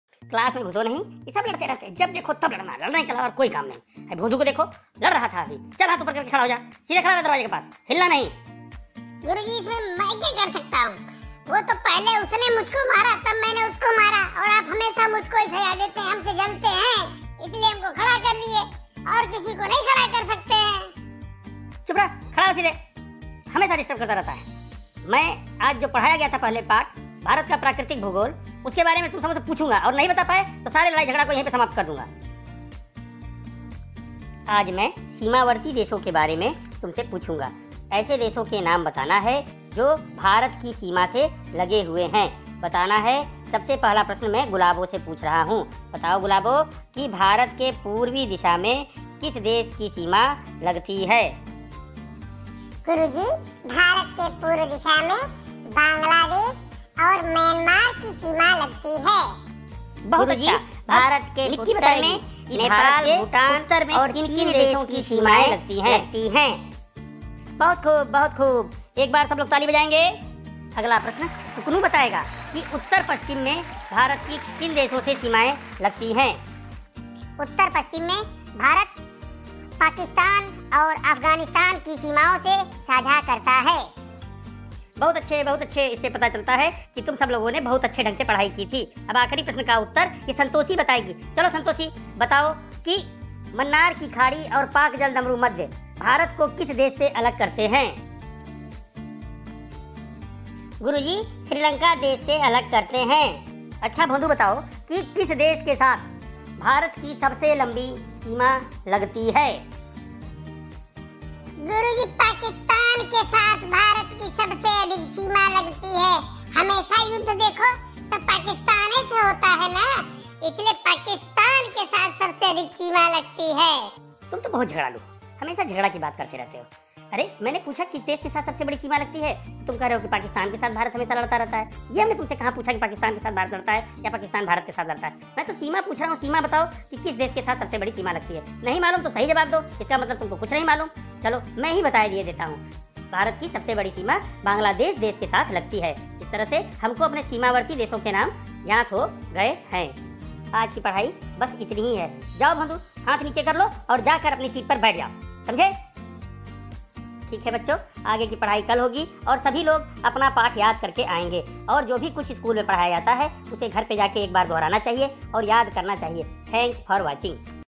BHARAT KE PADOSI DESH
In this movie children👶👧👦 know about nebouring contries of iIndia. this help for all classes. It is a cartoon movie